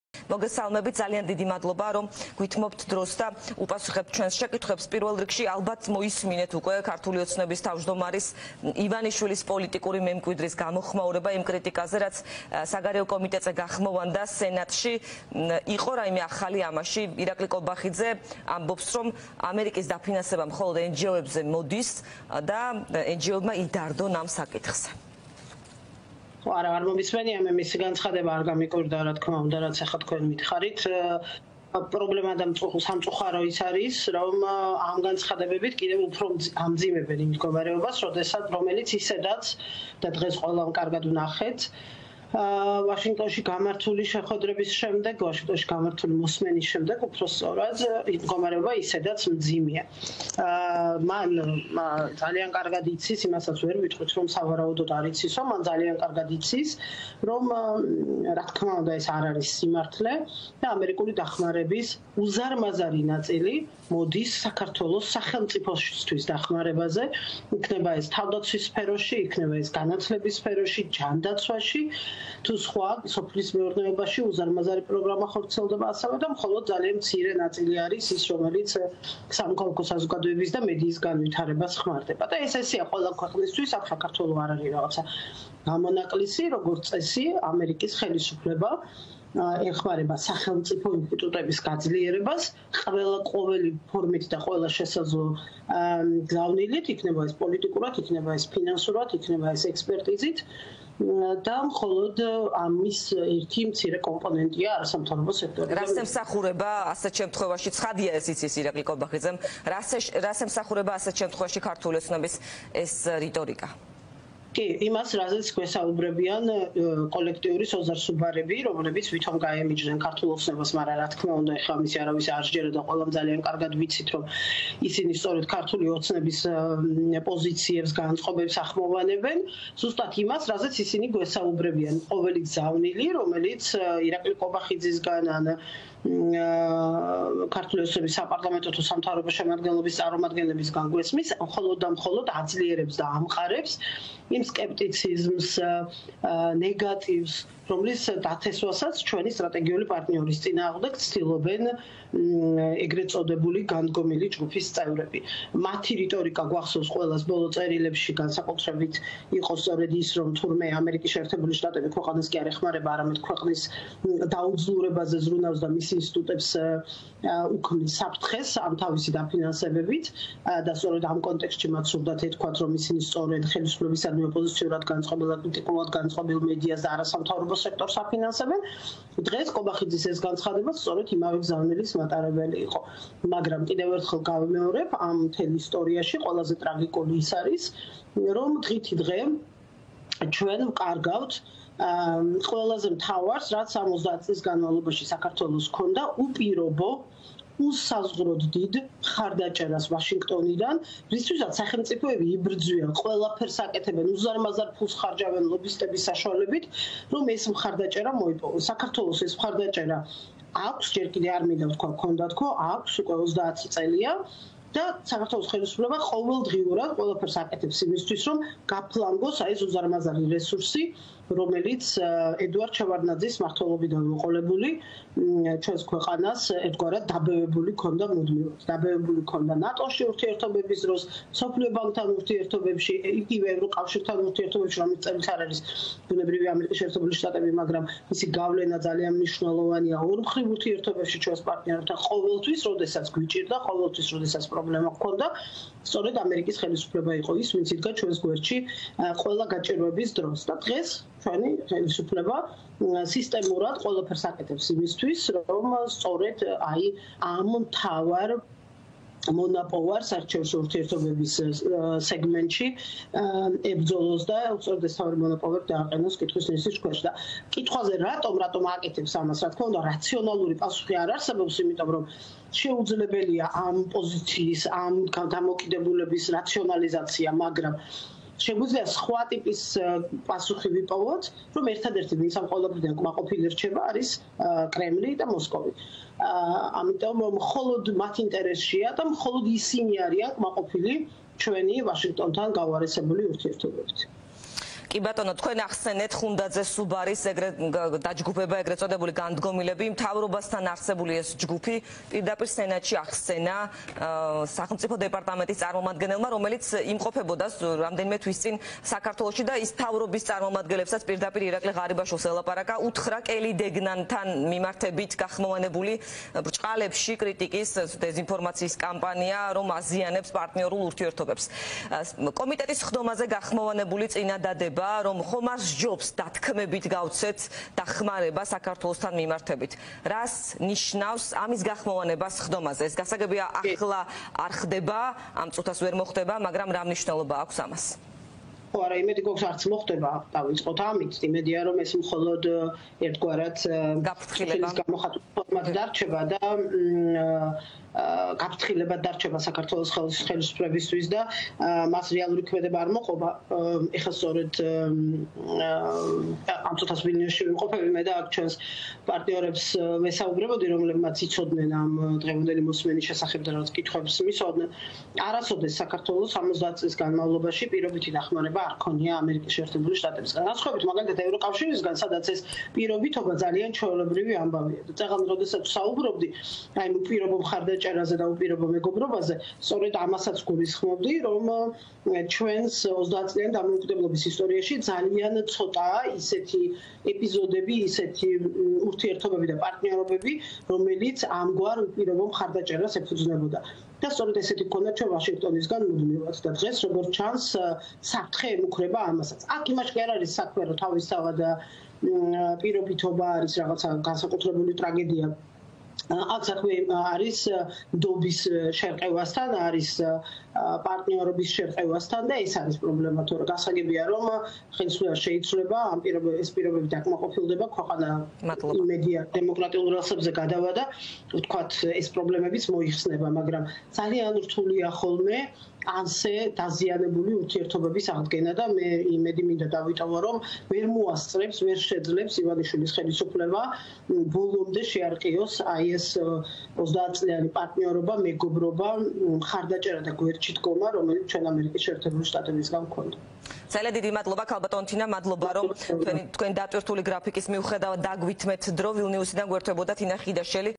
„ჩვენ ვკარგავთ უპირობო, უსაზღვროდ დიდ მხარდაჭერას ვაშინგტონიდან“ - ხიდაშელი
▪️  „ჩვენ ვკარგავთ უპირობო, უსაზღვროდ დიდ მხარდაჭერას ვაშინგტონიდან, რისთვისაც სახელმწიფოები იბრძვიან, ყველაფერს აკეთებენ, რომ ეს მხარდაჭერა მოიპოვონ“ - თინა ხიდაშელი